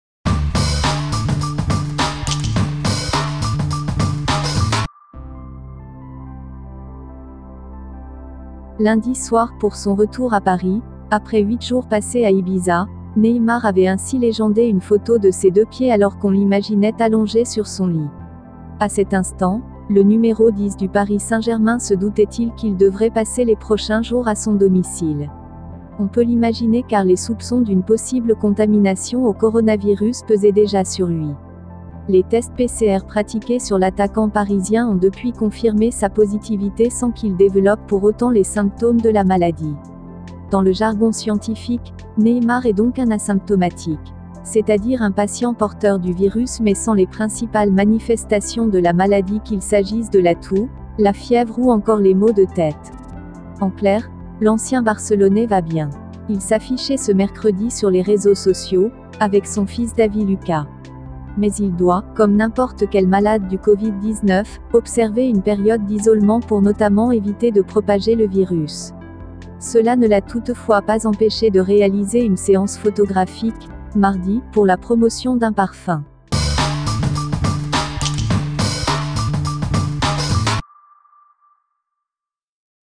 Neymar: contrôlé positif au Covid-19
Contrôlé positif ce mercredi au coronavirus, Neymar ne présente pas les symptômes de la maladie. Cela n’empêchera pas le meneur de jeu brésilien d’observer une période d’isolement.